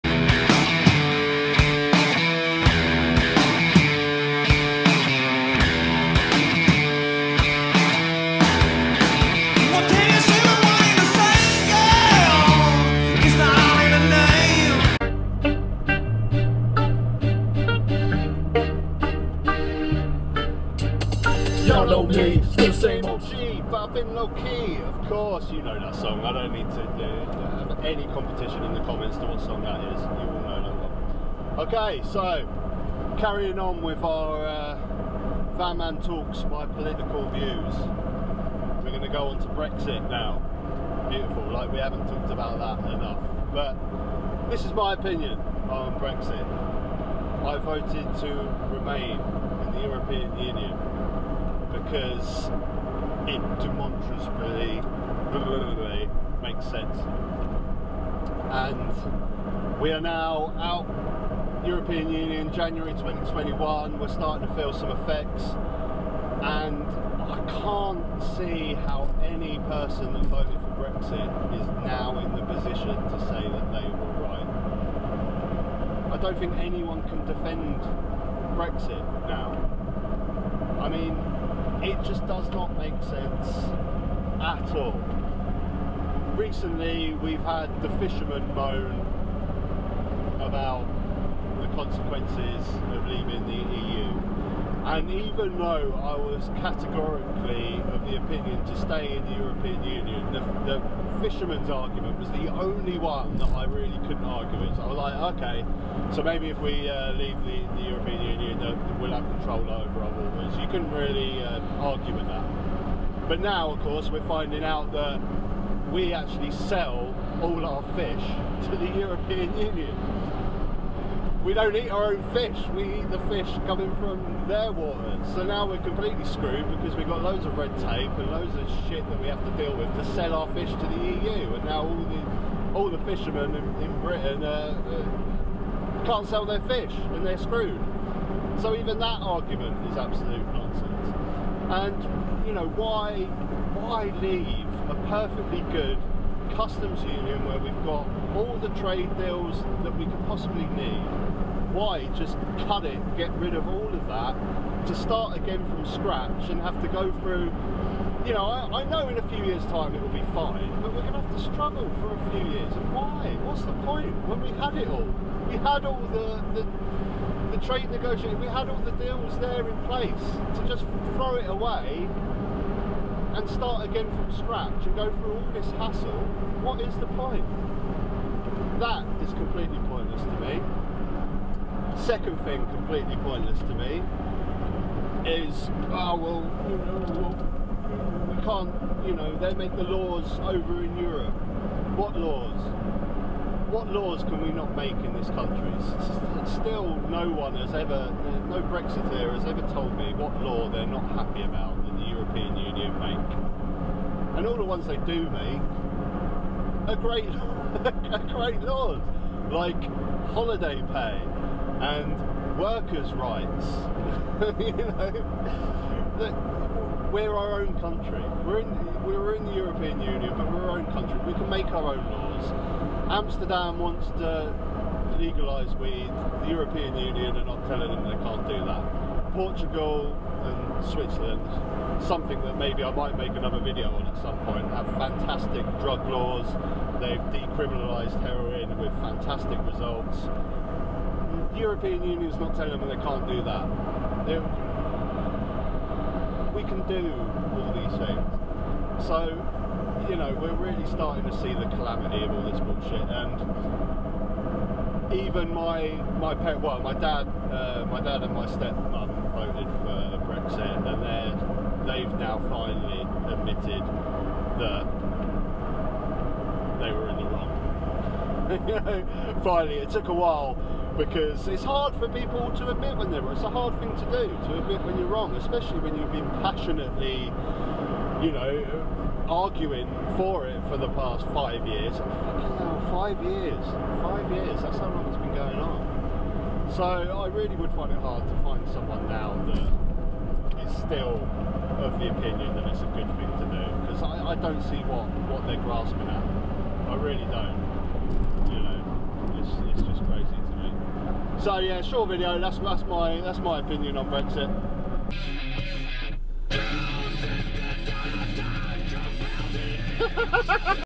van man talks brexit part 1 (brexit explained)
Van man talks Brexit, i give my opinion on the current state of brexit in the UK

I talk about how Brexit makes me feel about being British and the regret that some people are having, the creation of Brexit expats Global news and EU news Brexit spain.
Also a lot of talk on Wion news and Ursula von der leyen and Dominic Raab recent outburst on DW news